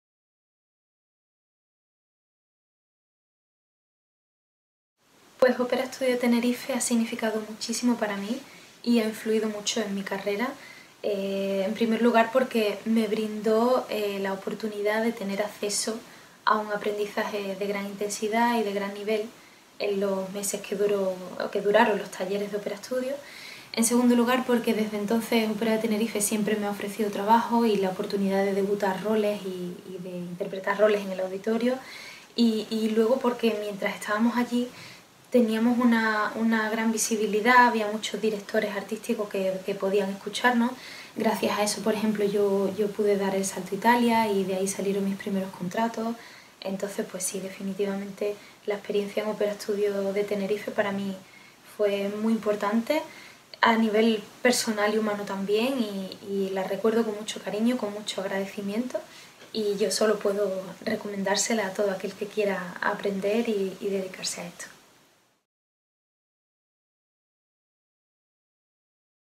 Leonor Bonilla cuenta su experiencia en Opera (e)Studio
La soprano Leonor Bonilla nos cuenta cómo influyó en su carrera profesional su paso por Opera (e)Studio.